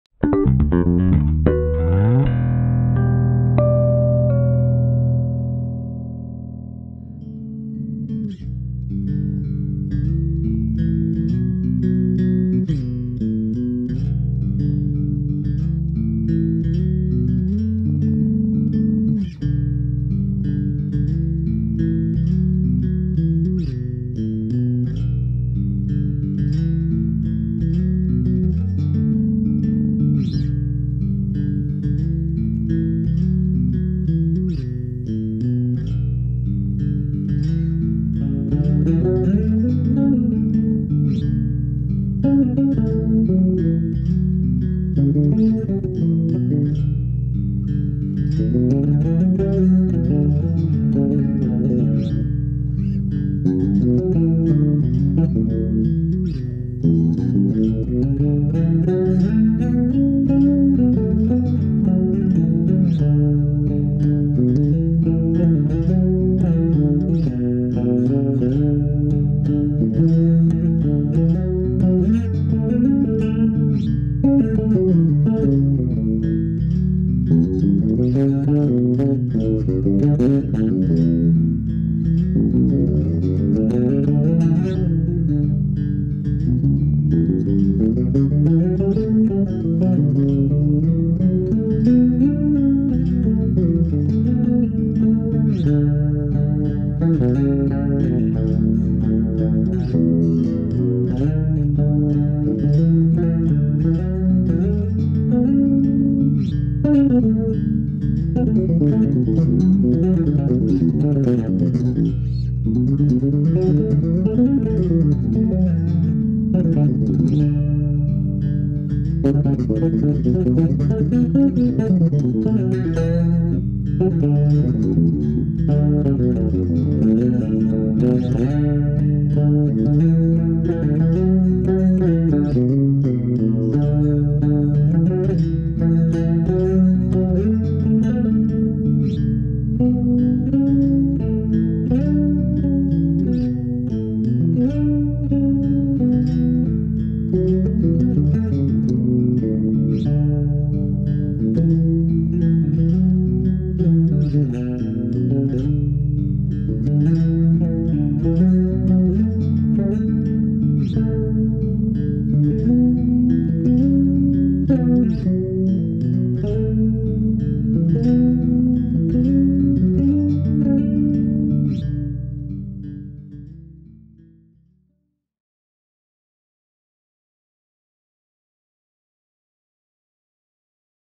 Studio Sessions ft. Matt Ramsey: Live Looping Improvisation and Bass Solo (Legacy 800 + NEO IV 410)
GK artist Matt Ramsey came by the studio while on tour with Kirk Franklin to check out the Legacy 800 and NEO IV 410. Part 2 has Matt creating a bass loop and soloing over it live. He uses a chorus pedal for the soloing.  This is one take, completely improvised.  Matt has the contour engaged and the low-mid completely off. Everything else is set flat.

Recorded by Victor Nash at Destination! Universe in Portland, Oregon
Shot and Edited by: Forest Gallien